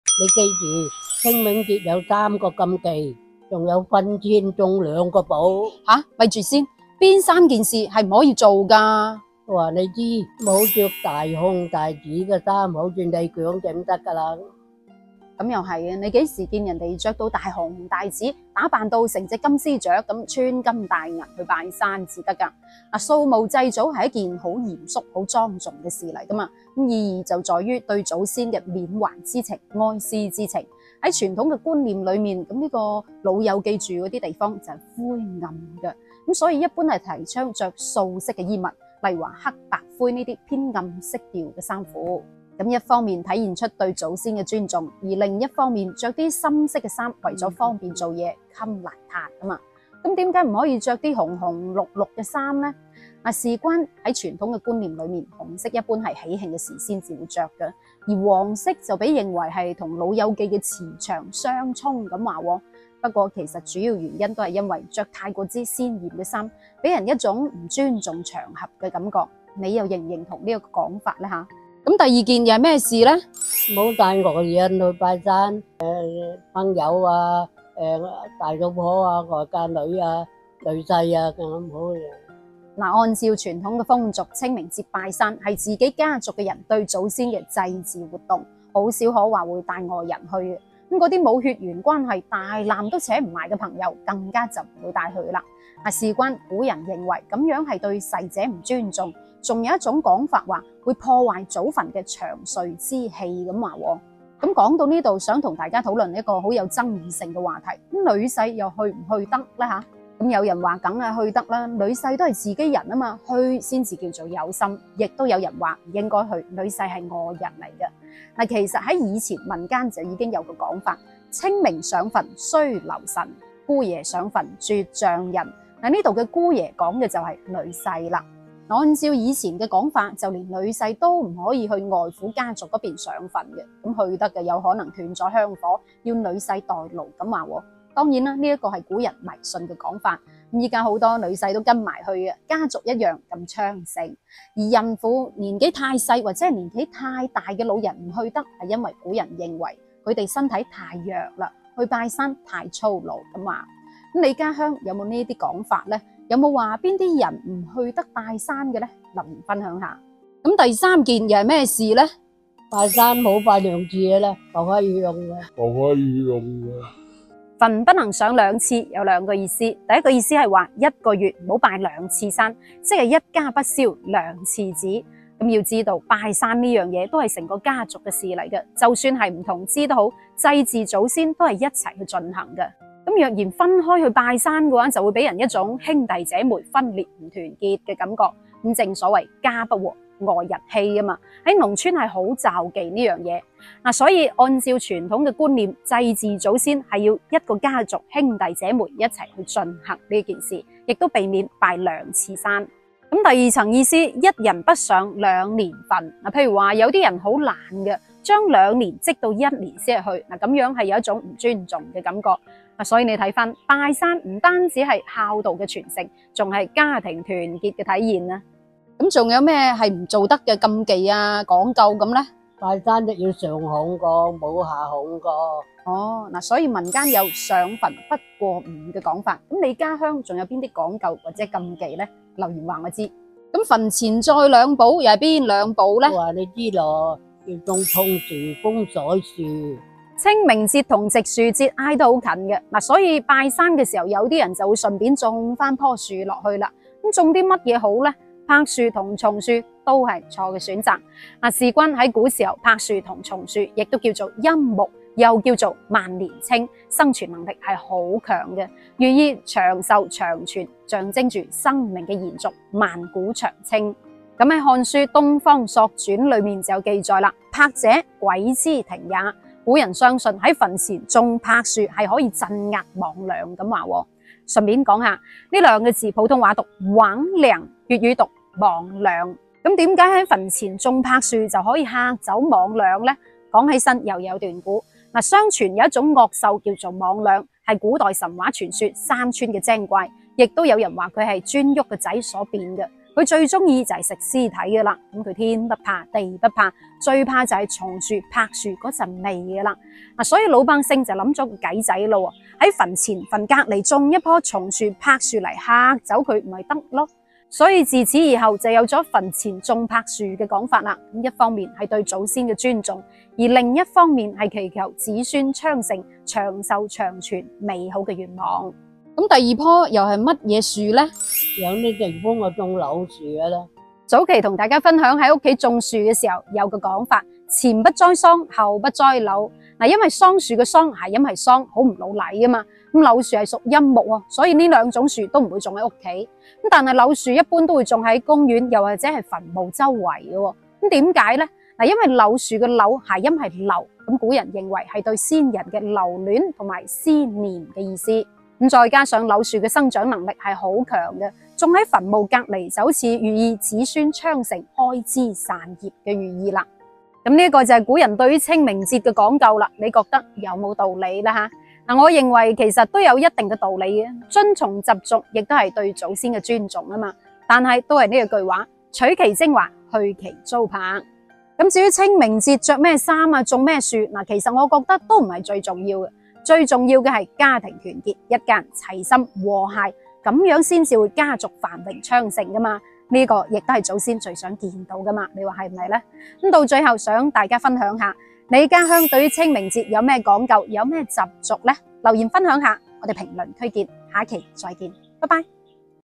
“清明上坟有三忌，别忘坟前栽两宝”，老一辈的讲究，图个顺心
粵語知識庫：

哈嘍，大家好，這裡是大臉妹
我是來自廣東的女子
講白話撐粵語
分享不為人知的粵語冷知識
每日一更